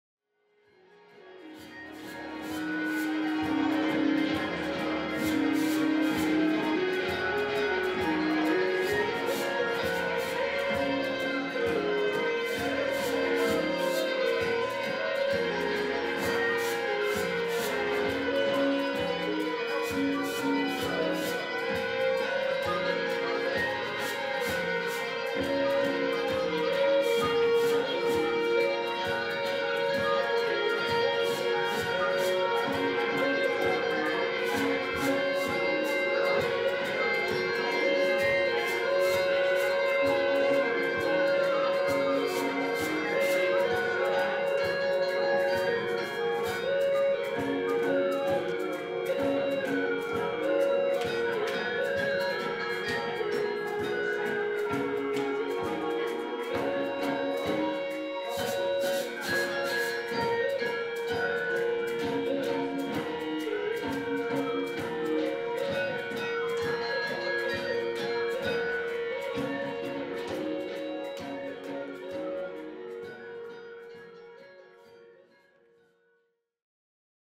Shallal Studios Underworld Bat Sound and Movement
This short film is a window into our creative, collaborative, multisensory sound and movement workshop inspired by bats as part of our Underworld project 2021-22. Shallal Studios encourages self-led creative practice and increases access to the arts.